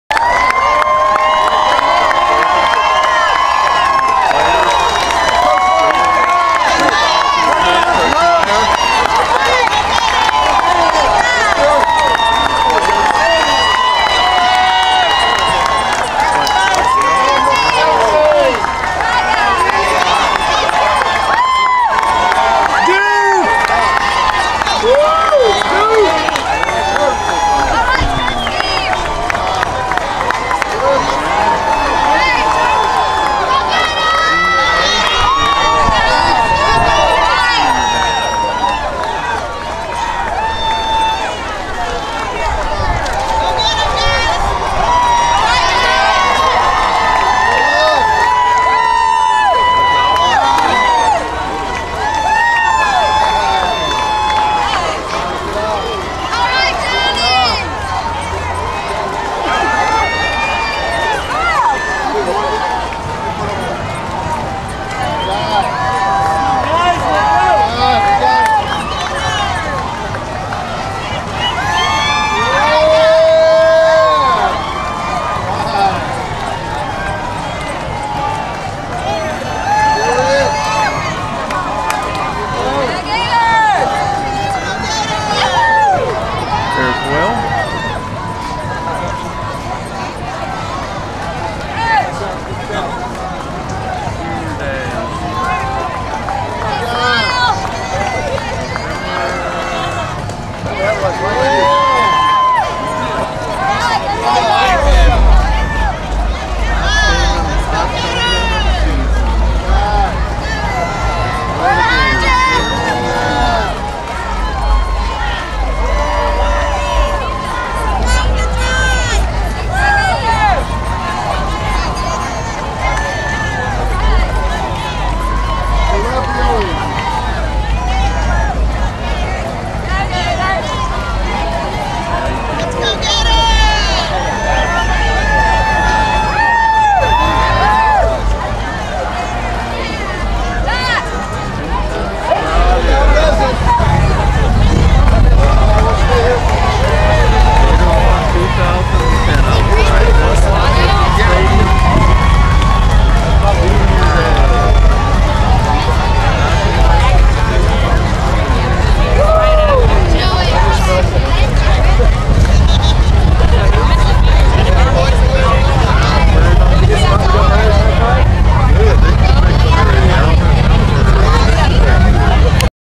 UF Football Gator Walk - Senior Day 2010 entire team (Gainesville, Florida)
UF Football Gator Walk - Senior Day 2010 entire team including Coach Urban Meyer (Gainesville,  Florida) 11/20/2010